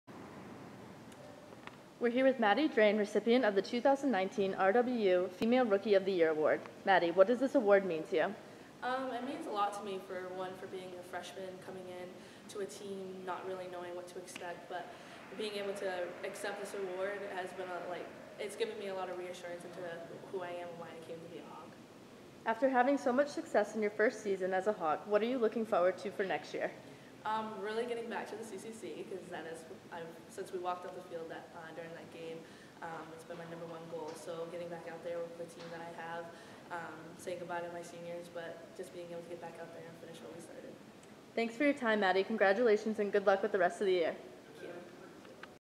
Madi Drain - 2019 RWU Female Rookie of the Year
Madi Drain talks about being named the 2019 RWU Female Rookie of the Year